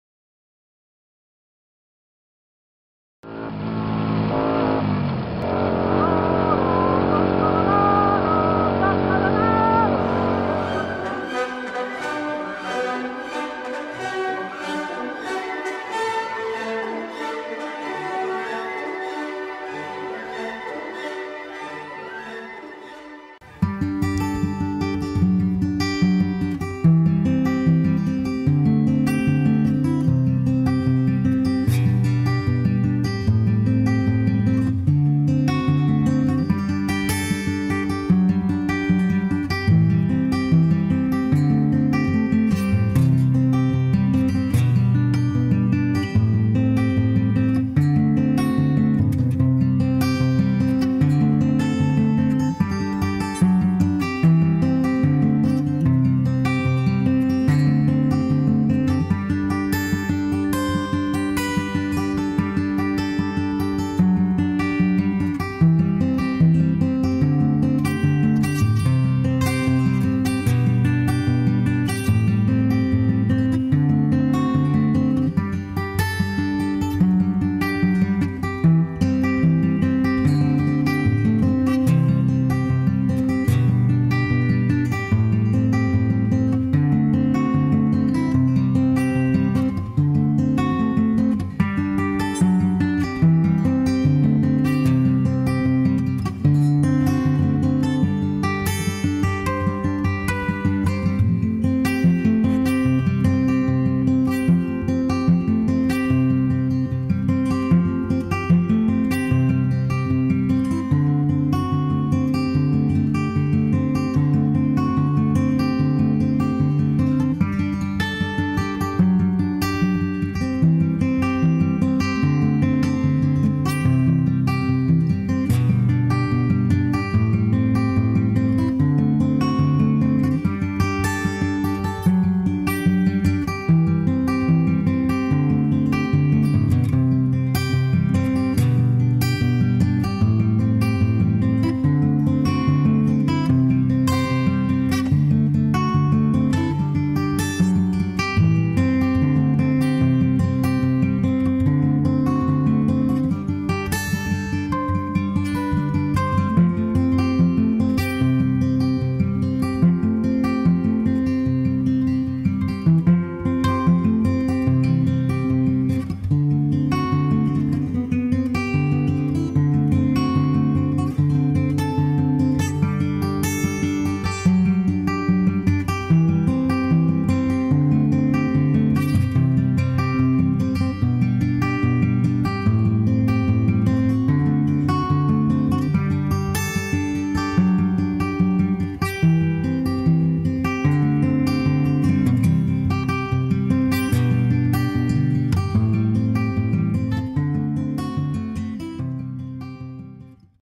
Wymiana oleju w wale kardana - krok po kroku - final drive oil change - Honda Shadow
Wymiana oleju w wale kardana - krok po kroku - wersja dla oporniejszych ode mnie (są tacy?). Obowiązkowo z psem pijącym kawę! Honda Shadow VT100 C3 Aero.